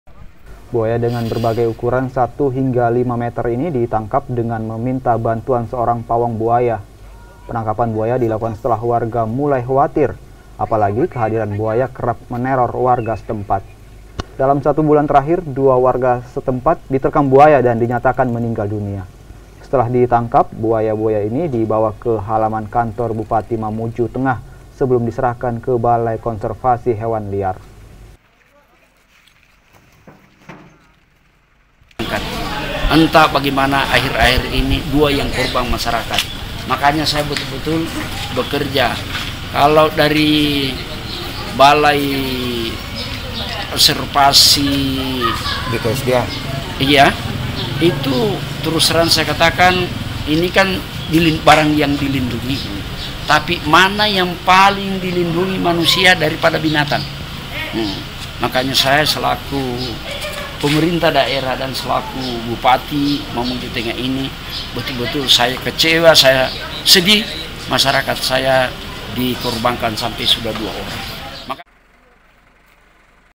Resahkan Warga, Belasan Buaya Ditangkap Warga
MAMUJU, KOMPAS.TV - Belasan buaya di pesisir sungai budong  budong  mamuju tengah ditangkap warga setempat . Langkah ini dialkukan setelah buaya  buaya itu mulai meresahkan warga sekitar .

Buaya dengan berbagai ukuran satu hingga lima meter ini ditangkap dengan meminta bantuan seorang pawang buaya . Penangkapan buaya dilakukan setelah warga mulai khawatir  apalagi kehadiran buaya kerap meneror warga setempat .

Dalam satu bulan terakhir  dua warga setempat diterkan buaya dan dinyatakan meninggal dunia .

Setelah di tangkap  buaya  buaya itu di bawa ke halaman kantor bupati mamuju tengah sebelum diserahkan ke balai konservasi hewan liar .